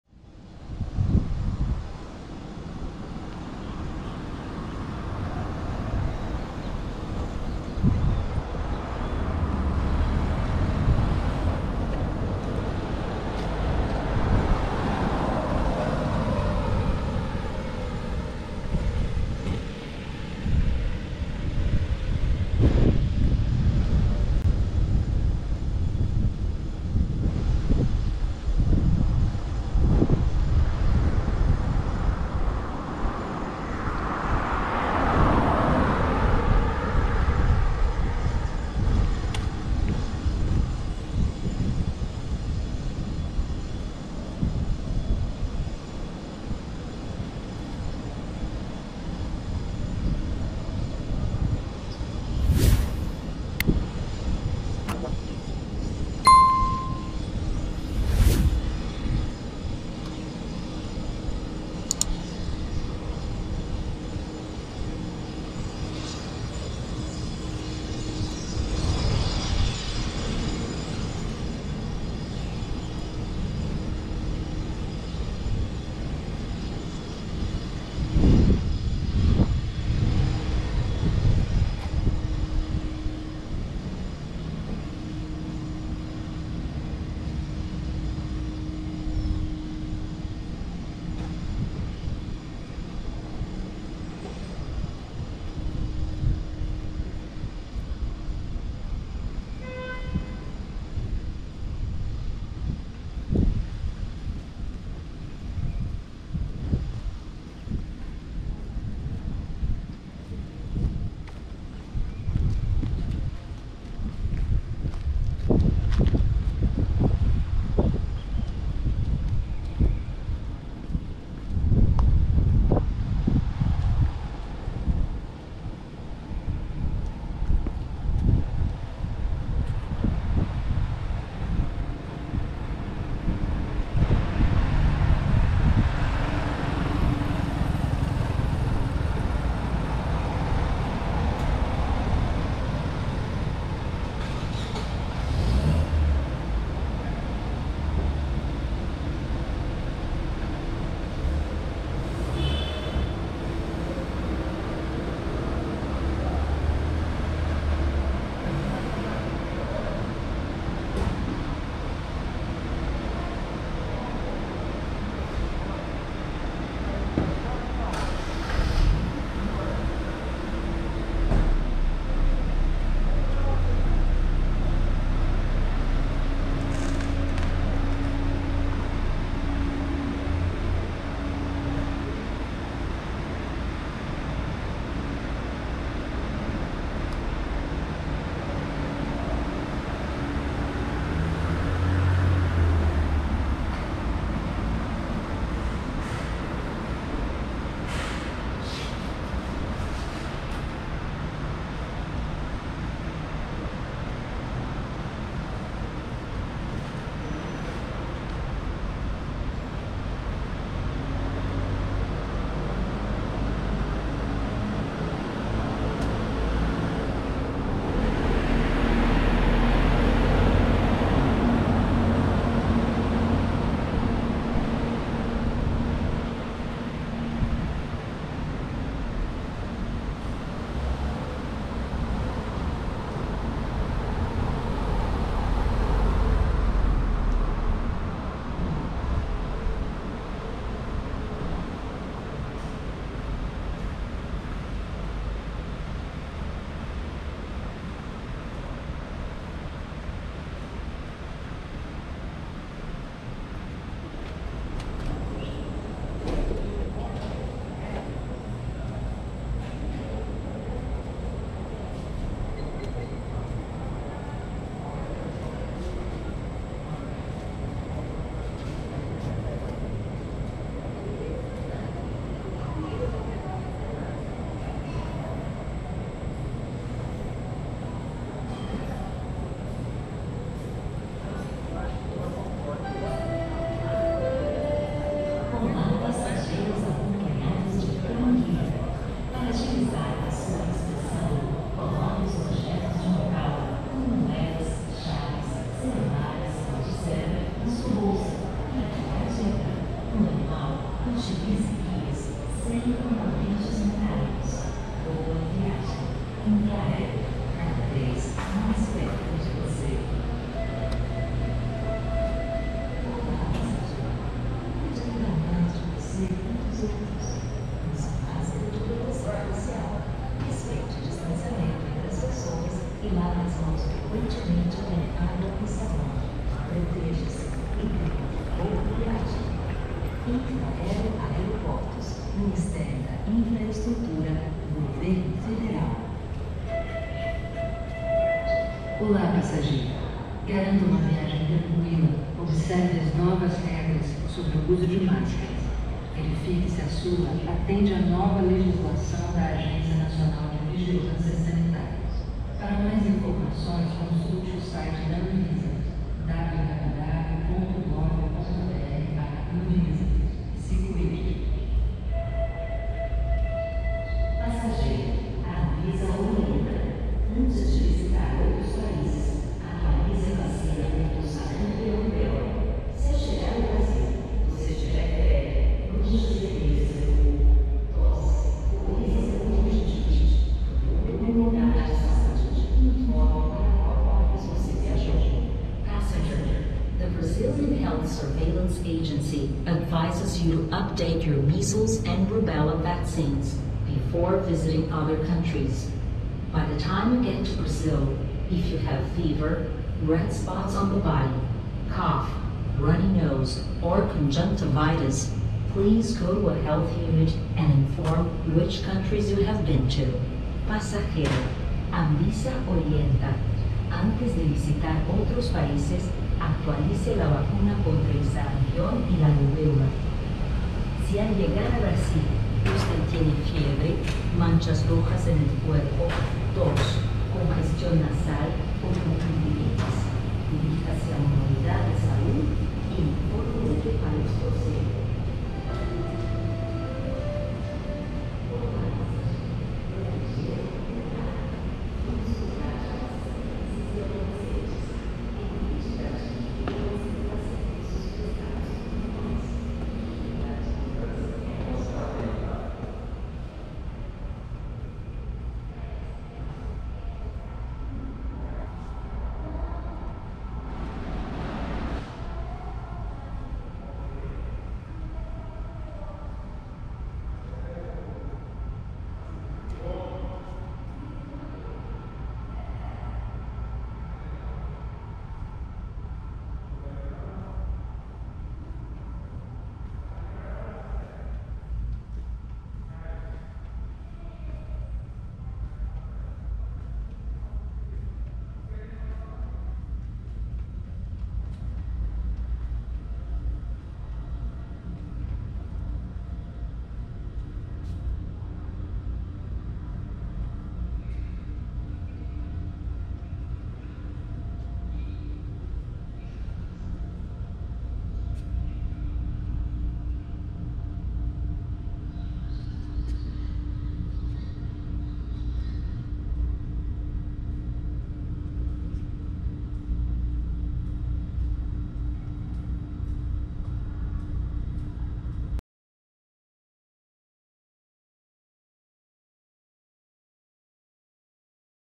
Conheça o Aeroporto de São Luís, MA, 15/02/2022 | Walking São Luís Airport
4k30fps -  Caminhando pelo Aeroporto de São Luís, Maranhão 15/02/2022
Recomendamos assistir em tela cheia para uma maior qualidade do vídeo.

00:00 Av. dos Libaneses
02:13 Entrada Principal - Terminal de Passageiros
04:08 Terminal de Passageiros
05:51 Ampliação do Terminal de Passageiros
07:18 Terraço Panorâmico

Walking São Luís Airport, Maranhão, Brazil 15/02/2022